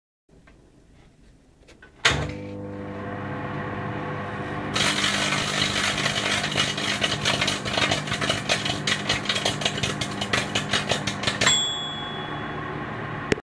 CDs In Microwave 2/2
Not much to say. Had some CDs to destroy, thought this would be the best way to do it. See video #1